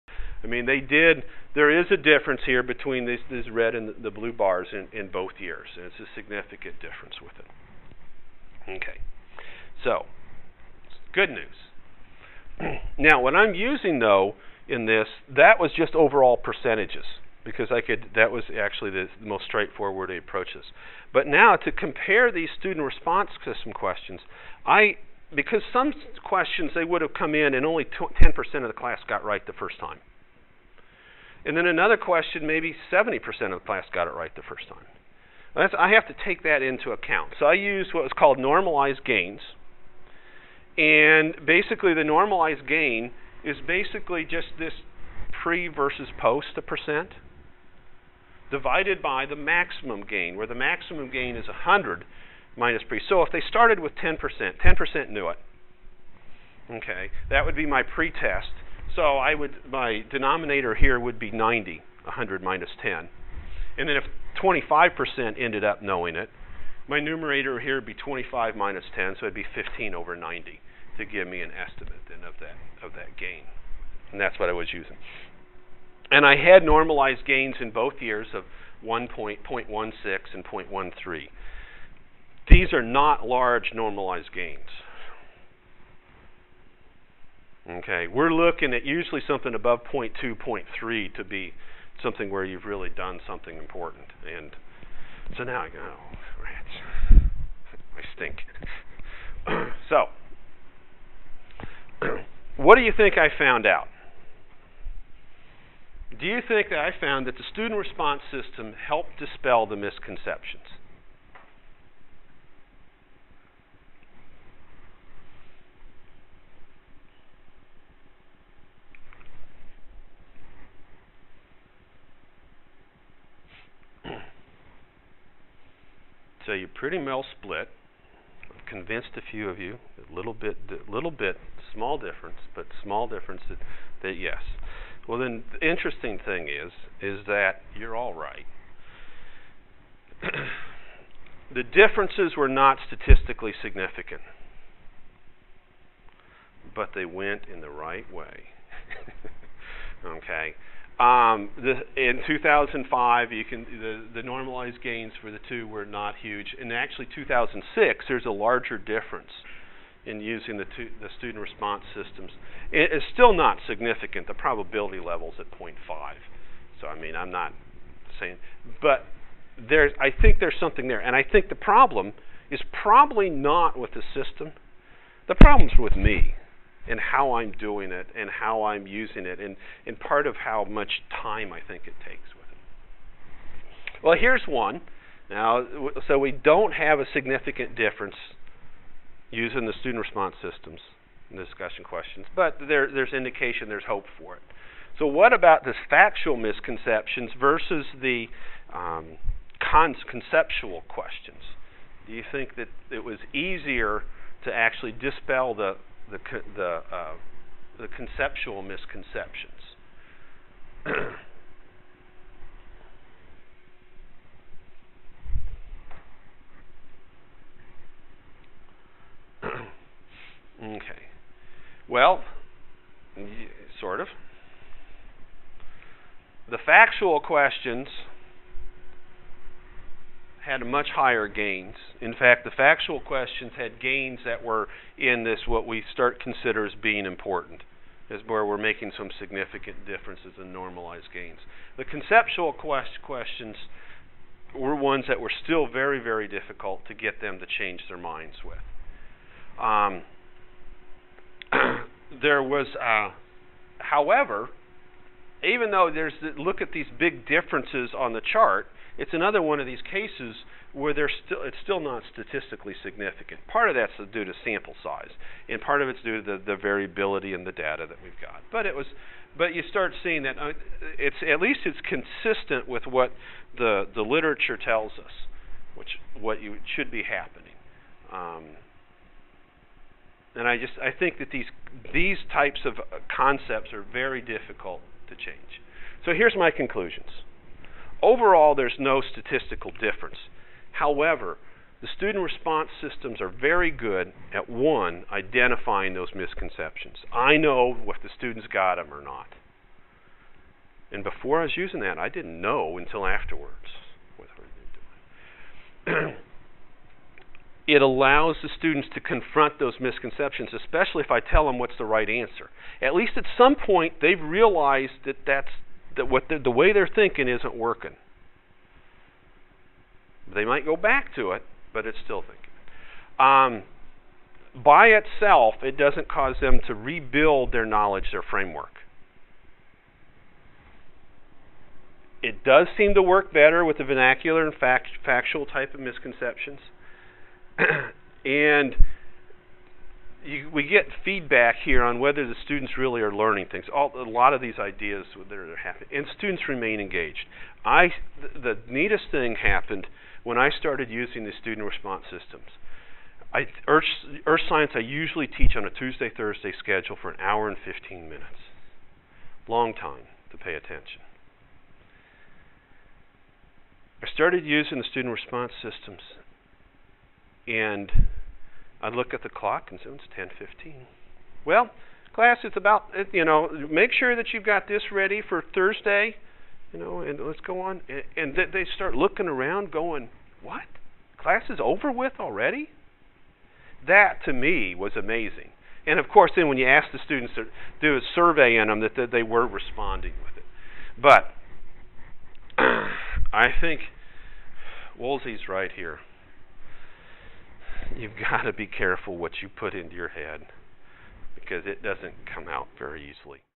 Clicking Misconceptions Away: part 4
Click misconceptions away by using a student response system in earth science.